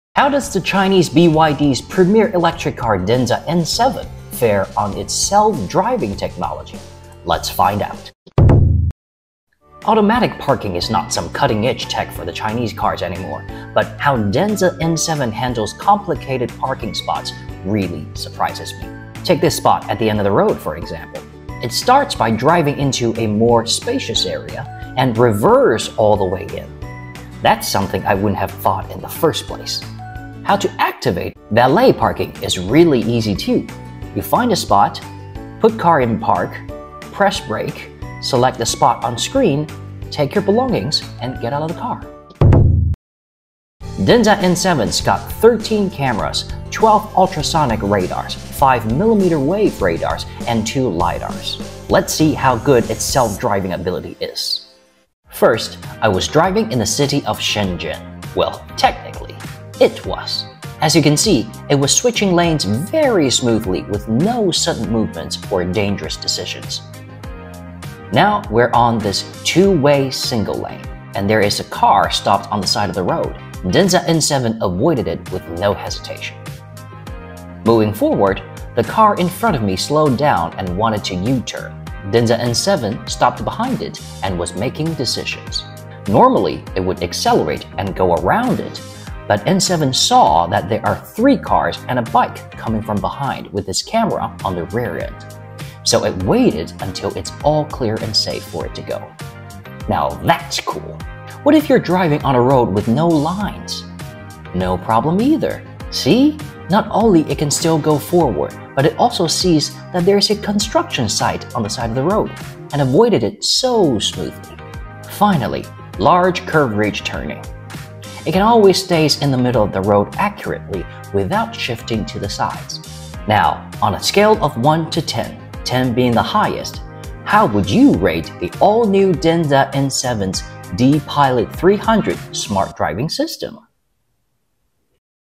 Better than Tesla's FSD 12? Chinese premiere smart driving system review!
Better than Tesla's FSD 12?

Watch me test the Chinese premiere smart driving system——Denza's DiPilot 300 in the city of Shenzhen!